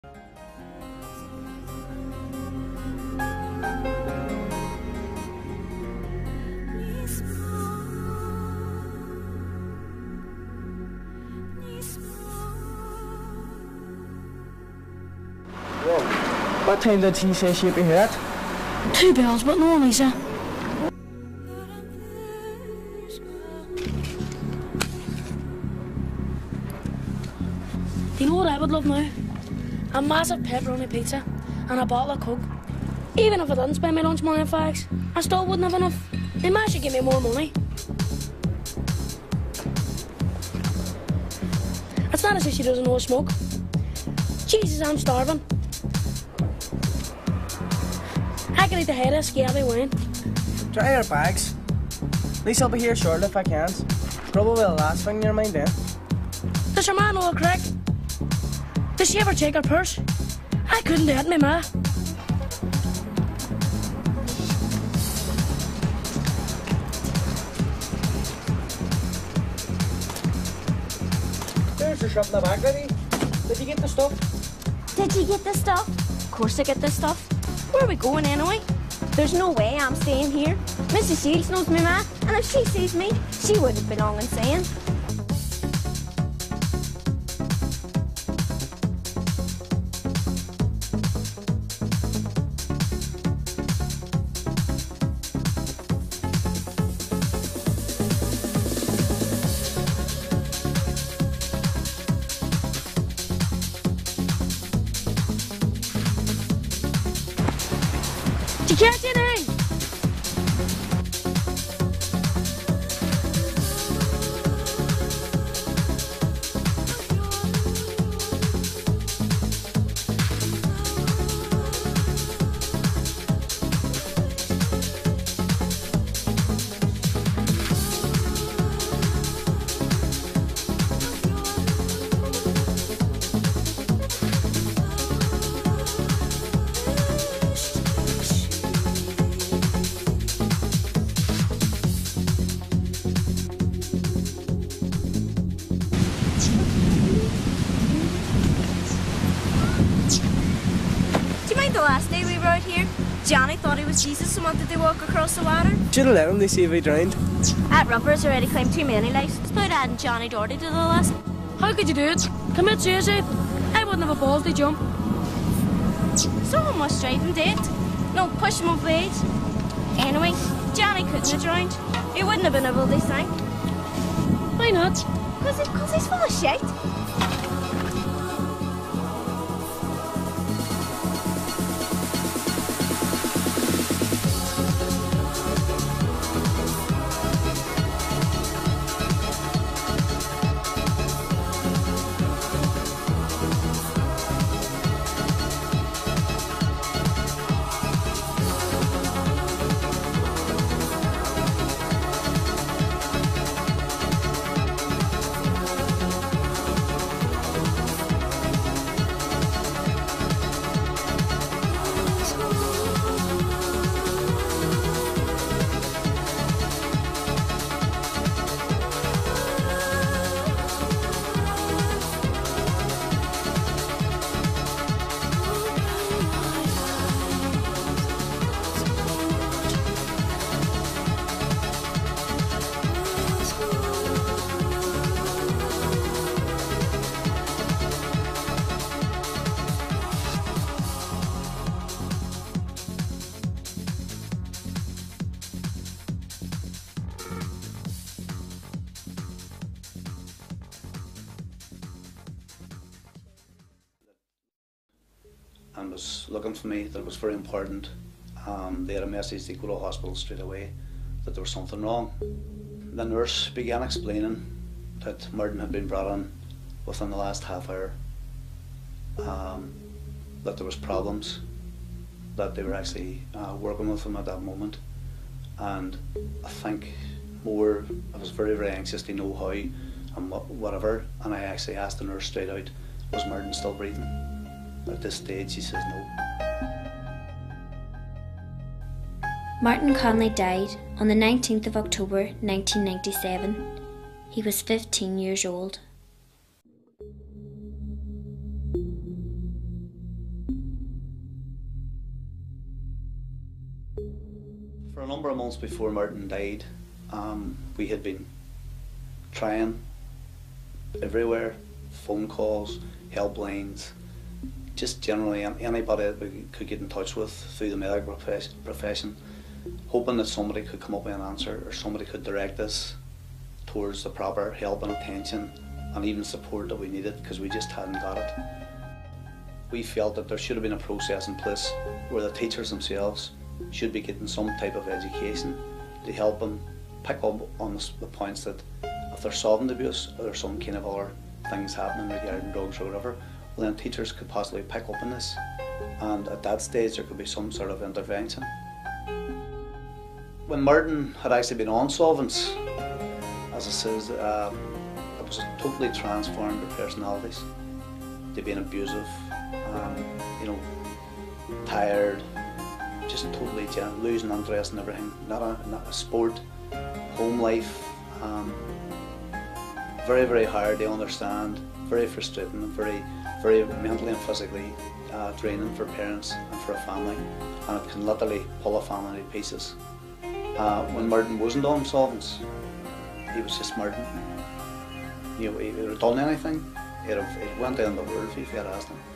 Solvent Abuse - Derry Film
Based on a true story - Martin Connelly (MAC) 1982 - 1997

Solvent abuse became a serious problem among young people in Derry's communities during the mid-1990's.

Mac was born on the 29th January 1982 in Derry and was much loved by his family and friends. He attended Rosemount P.S. and then St Joseph's secondary school and was an exceptionally talented soccer/gaelic player heading towards a place on the Doire minors team. He was also a great runner and swimmer and was on track to becoming a professional athlete. "He was kind and would have went to the end of the earth for you if you asked him too".

Mac's life slowly began to change. He became moody, tired and abusive and lost interest in sports, school work was going downhill. His family were worried and tried to help. After his mum met with his teacher she was told he was experimenting with solvents. "We were distressed and tried to get him help. We talked to his school, medical professionals and called several helplines but did not get the help we needed."

On the 19th October 1997 I got a call to go to the hospital immediately. When I arrived doctors were working on Mac. I asked "Is he still breathing?" to which the reply was "no".

Martin Connolly Jnr died on the 19th October 1997 as a result of volatile substance abuse. Unfortunately this was not the first death in the Derry area. The community realised that a response was needed and needed now. As a result the Divert Project was founded.

"Drug and or alcohol use is not cultural nor does it distinguish between classes – it can impact on your life or destroy it completely. People can be experimenting and everything is ok then someone can just die."